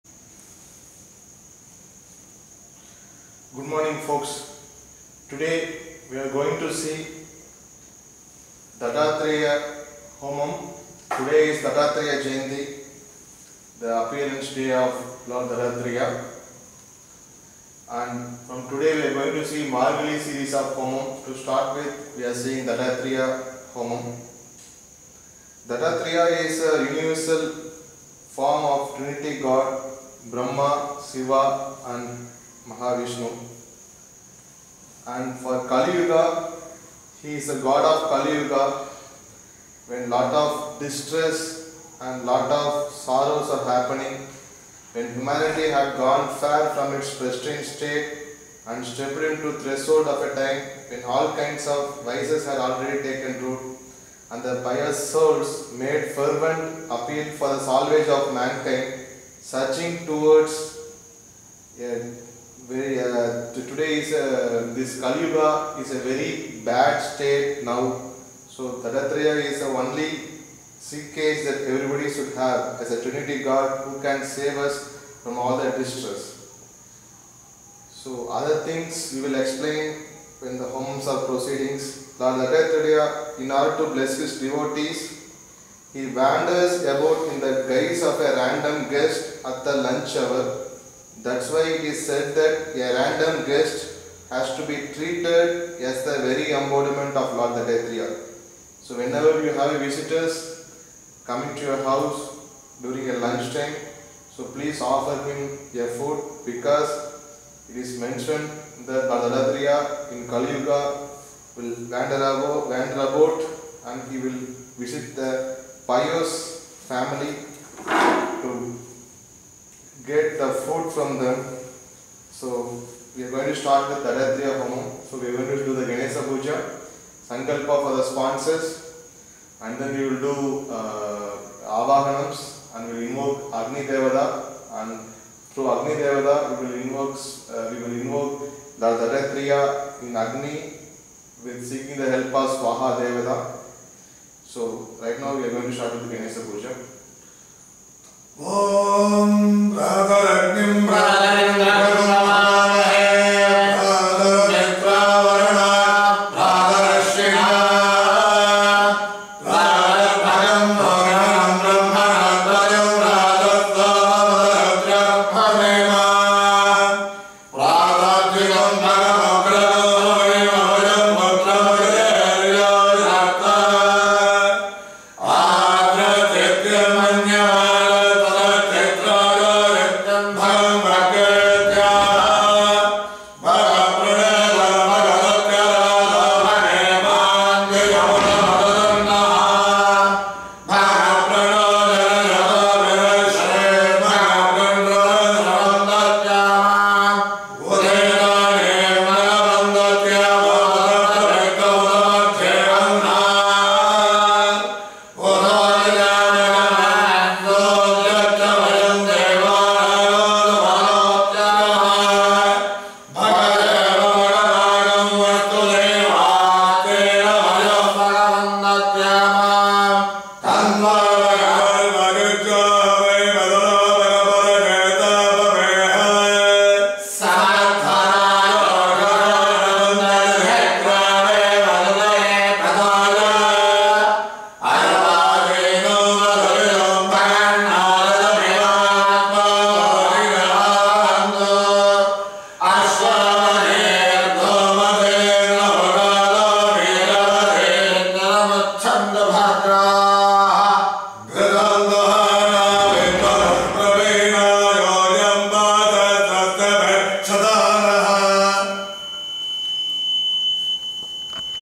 Lord Dattatreya Homam Part 1
|| I bow to the son of Atri, who has six hands and whose lowest two hands have maalaa and kamandalu, middle pair of hands hold damaru and trishool and top two hands have holy Shankh and Chakra ||

 Dattatreya is a powerful trinity form of Brahma, Vishnu and Siva. The name Dattatreya can be divided into two words - "Datta" (meaning given) and "Atreya" referring to the sage Atri, his physical father.